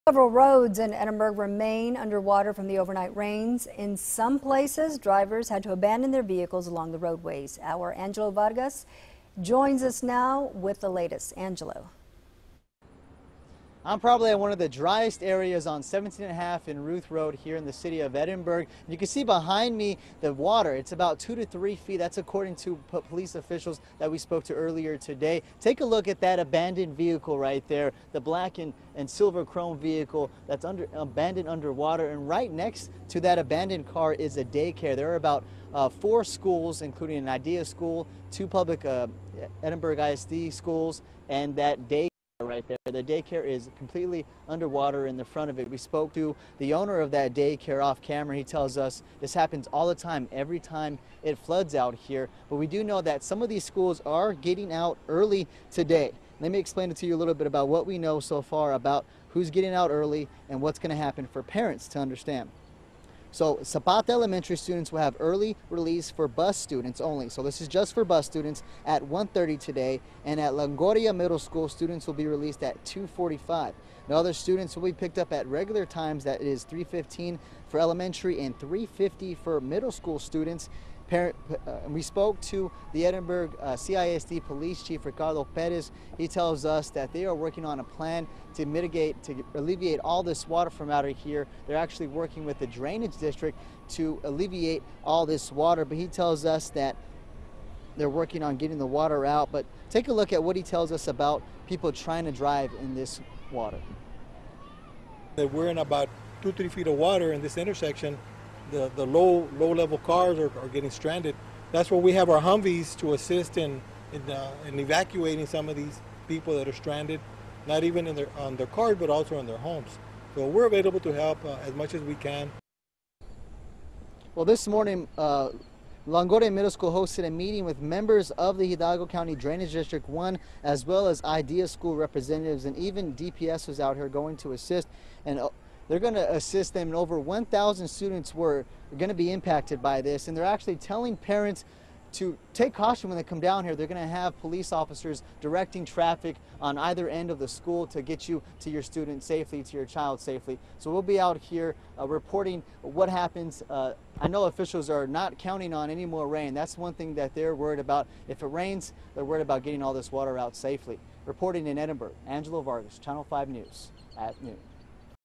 Early Dismissal in Several Valley Schools Due to Flood
Several school districts are cutting class short and dismissing students early due to flooding.